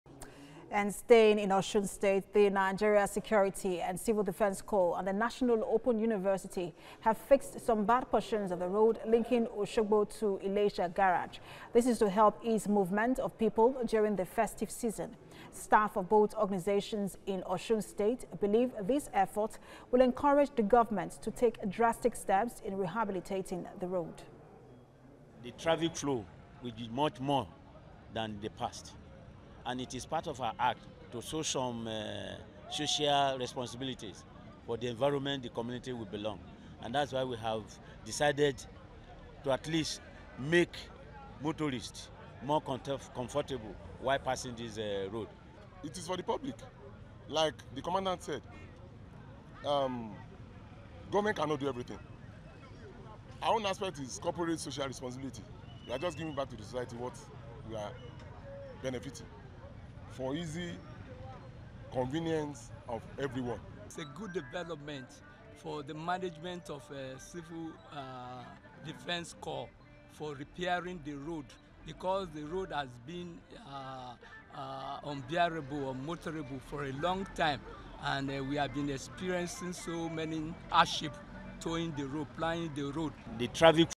NSCDC, NOUN fix bad portions of road linking Osogbo to Ilesa
Officers of the Nigeria Security and Civil Defence Corps, NSCDC, in conjunction with National Open University of Nigeria, NOUN, have fixed bad portions of road linking Osogbo to Ilesa.